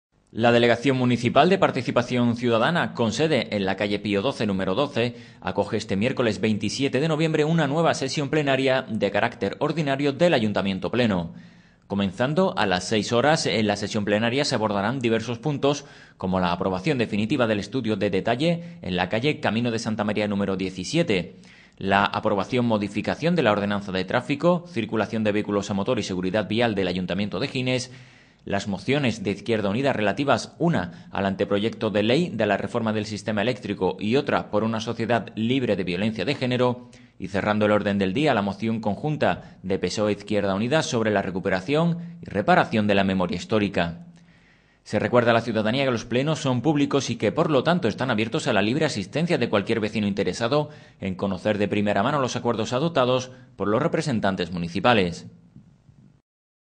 Este miércoles 27 nueva sesión plenaria del Ayuntamiento de Gines
La delegación municipal de Participación Ciudadana, con sede en la calle Pío XII número 12, acoge este miércoles 27 de noviembre una nueva sesión ordinaria del Ayuntamiento Pleno.